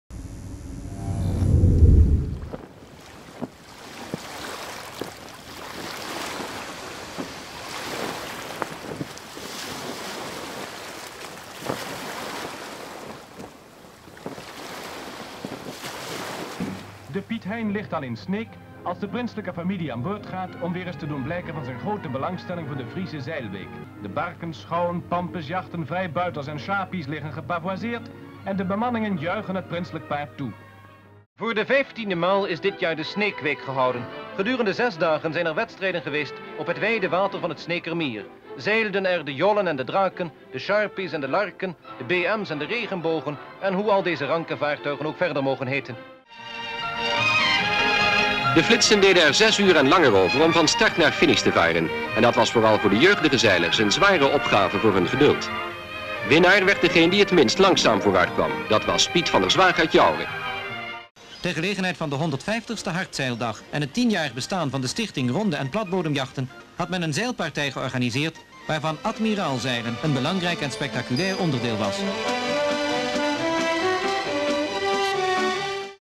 Sneekweek in beeld (1930 - 1965)
Tijdens de opening van de Sneekweek, op vrijdagavond 1 augustus, presenteert het Nederlands Instituut voor Beeld en Geluid bijzondere archiefbeelden van Sneek en de Sneekweek.